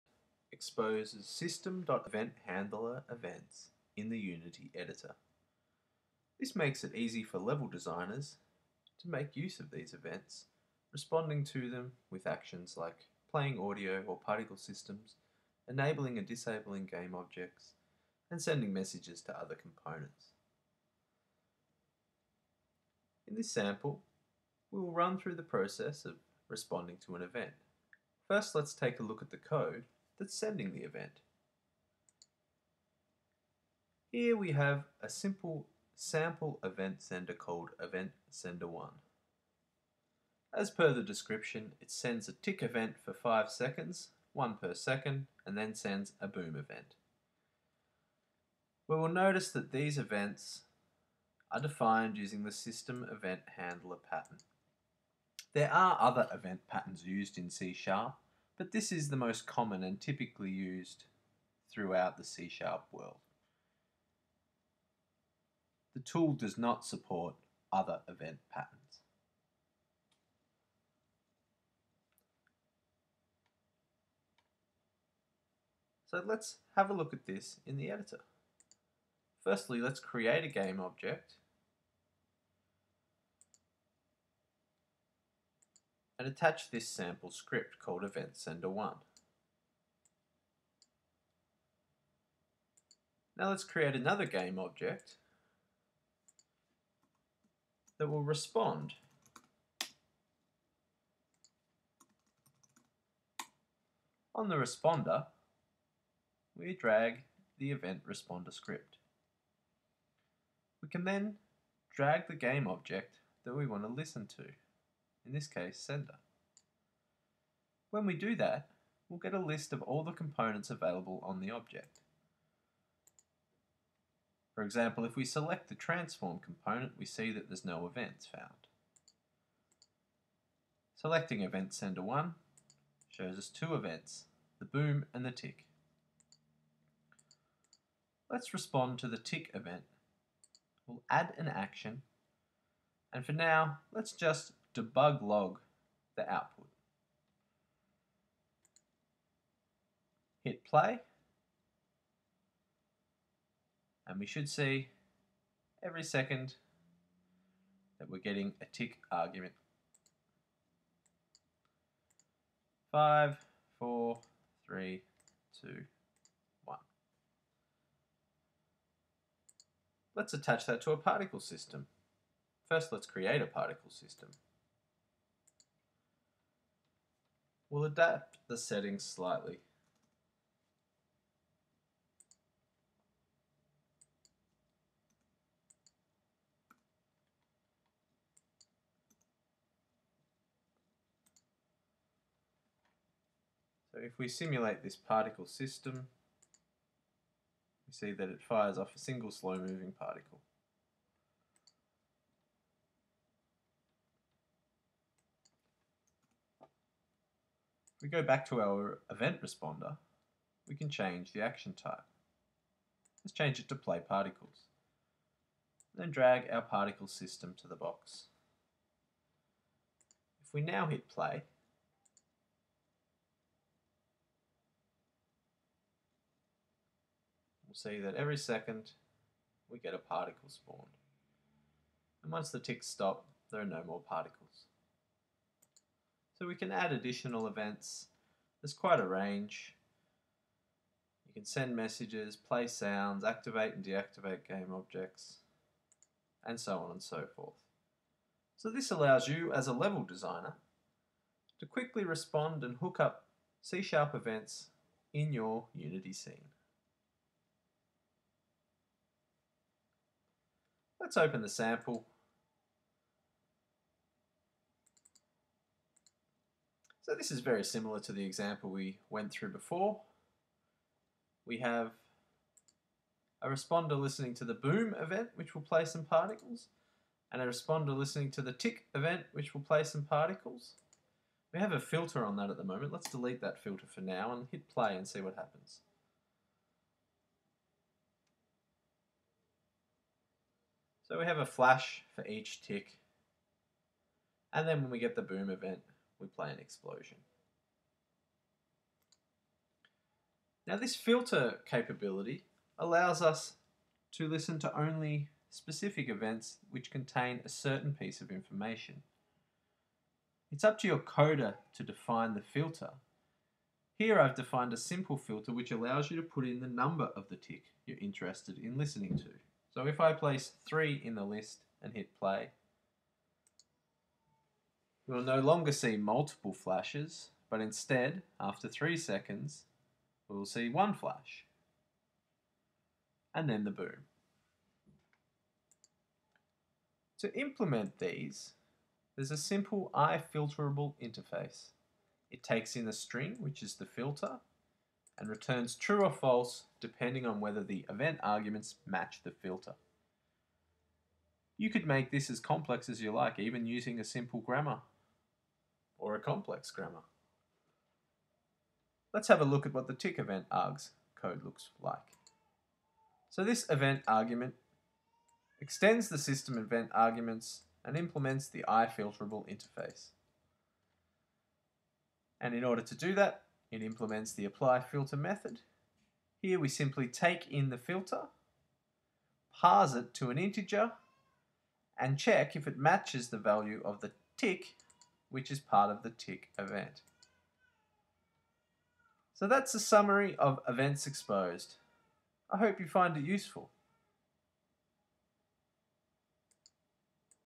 Events Exposed
Events Exposed allows you to listen and respond to c# events sent by objects in your scene hierarchy.